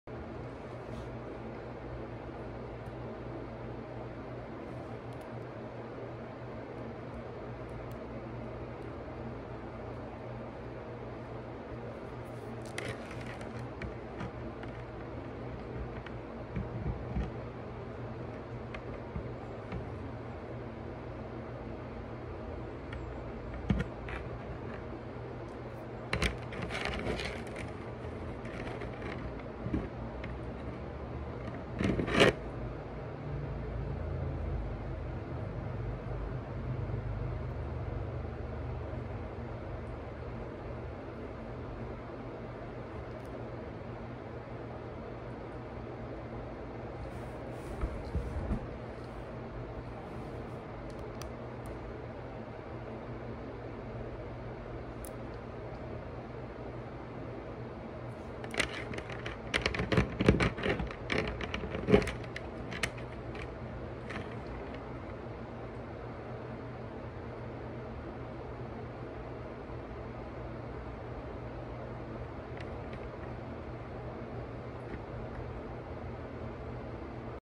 ACRYLIC NAILS LATEST 💅🏾 DID ON MYSELF PREVIOUSLY!
👋🏾🙂💖! Being slowly teaching myself how do my own 💅🏾. Since I’ve been having the desire to wear them again.

An I do plan on putting out more (videos) personal and nails videos.